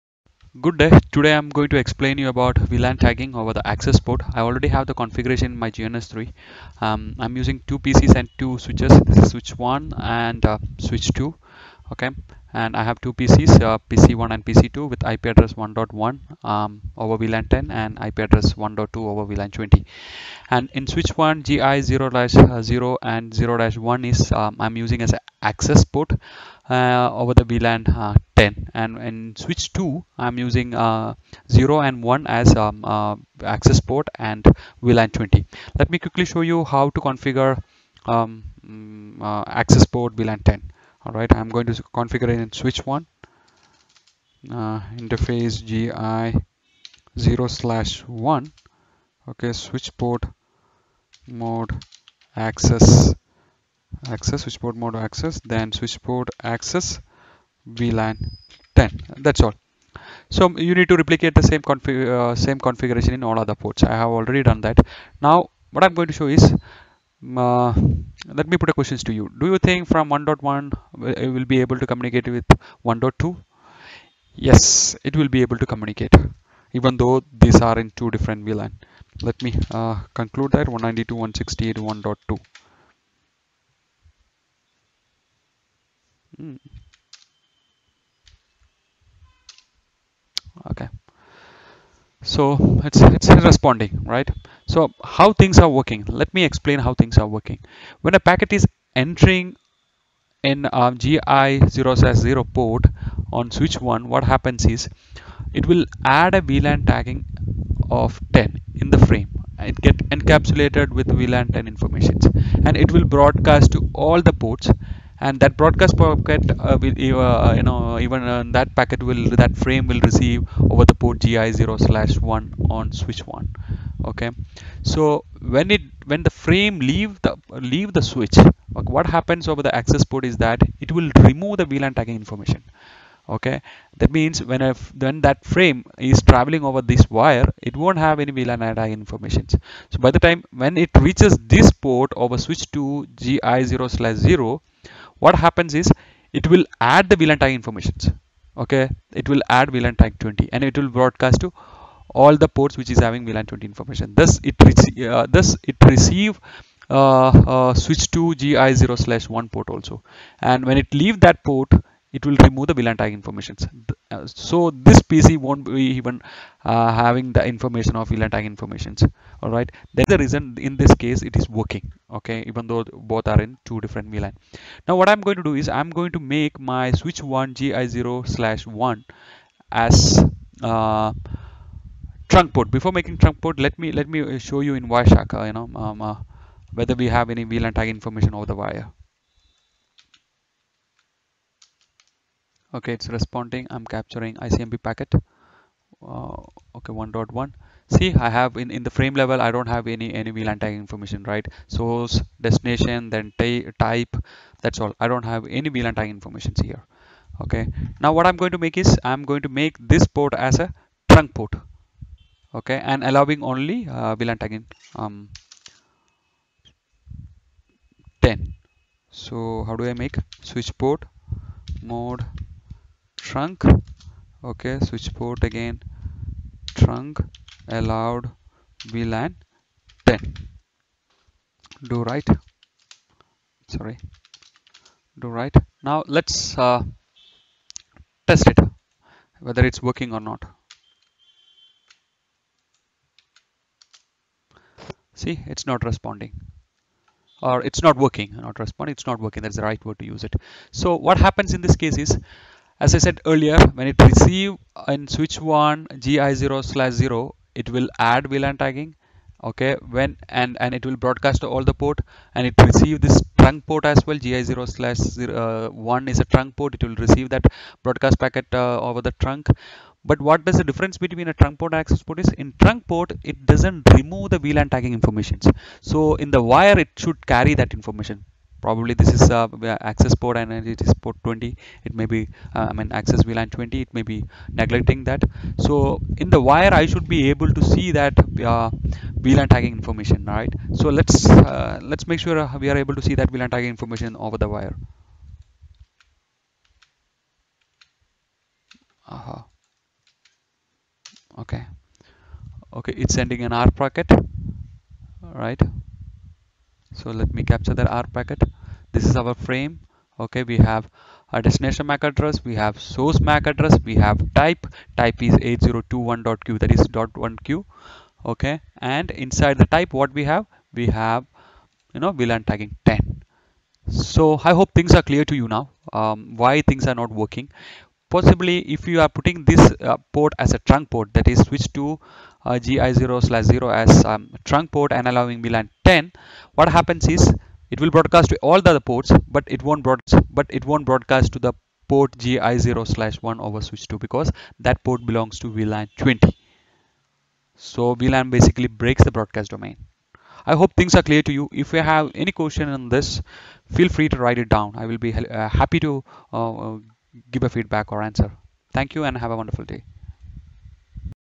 Interview Question - Communication between 2 different VLAN over access and trunk port
Scenario-based interview Question